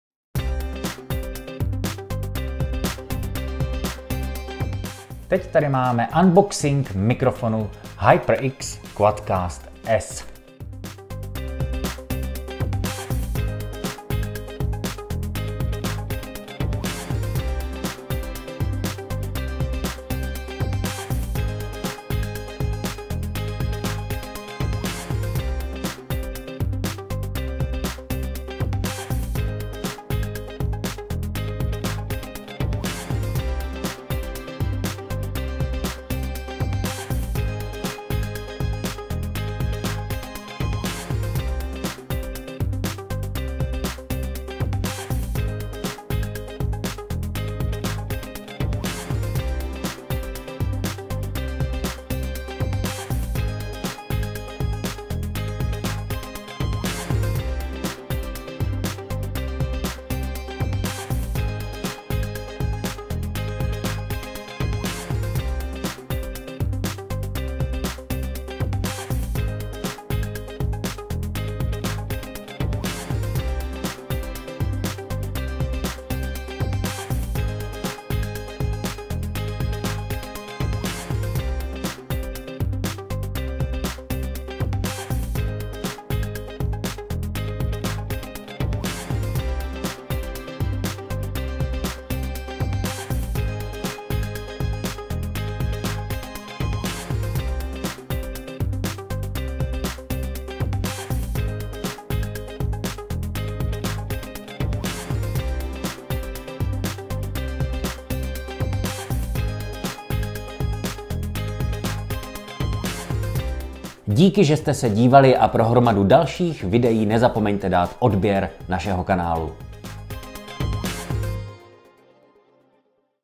Mikrofon HyperX QuadCast S | unboxing
HyperX QuadCast S je mikrofon, který ocení i pokročilí audio i video tvůrci, streameři nebo hráči a kreativci. Vybaven třemi 14mm kondenzátory a připraven na volbu mezi 4 polárními vzory (stereofonní, všesměrový, kardioidní, obousměrný) zajistí vysokou kvalitu zachycení zvuku v každé situaci.